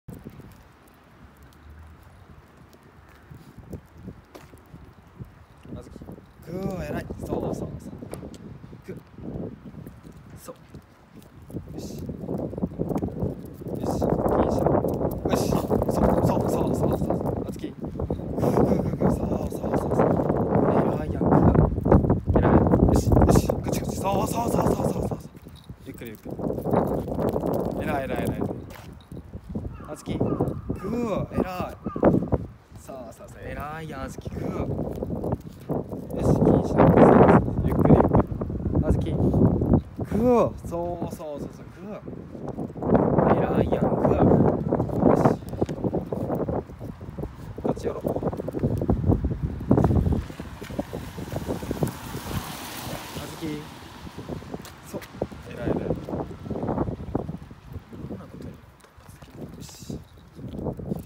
チワワの”あずきくん”と散歩トレーニング！
チワワの”あずきくん”と”散歩トレーニング”をしています！
外の環境に怖がって色々なものに吠えてしまうので、まずは名前を呼んでしっかりとこちらを見た時に褒めています！今回も散歩中に人を見て少し吠えたので、こちらは少し小走りをして注意をこちらに向けて練習中です(^_^)